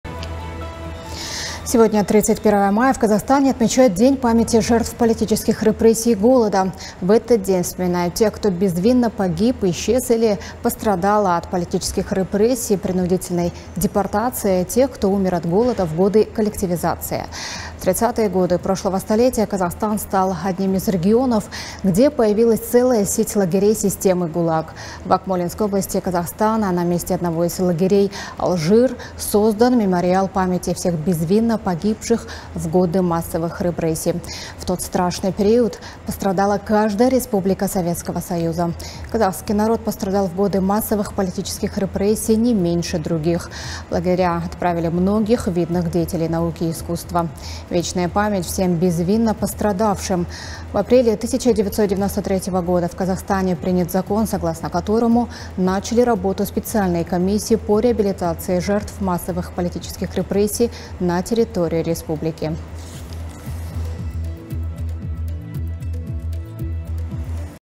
31 мая - День памяти жертв политических репрессий и голода в Казахстане
Сегодня 31 мая в Казахстане отмечают День памяти жертв политических репрессий и голода. В этот день вспоминают тех, кто безвинно погиб, исчез или пострадал от политических репрессий, принудительной депортации,тех, кто умер от голода в годы коллективизации. В 30-е годы прошлого столетия Казахстан стал одним из регионов, где появилась целая сеть лагерей системы ГУЛАГ. В Акмолинской области Казахстана на месте одного из лагерей «АЛЖИР» создан мемориал памяти всех безвинно погибших в годы массовых репрессий. В тот страшный период пострадала каждая республика Советского Союза. Казахский народ пострадал в годы массовых политических репрессий не меньше других. В лагеря отправили многих видных деятелей науки и искусства. Вечная память всем безвинно пострадавшим. В апреле 1993 года в Казахстане принят закон, согласно которому начали работу специальные комиссии по реабилитации жертв массовых политических репрессий на территории республики.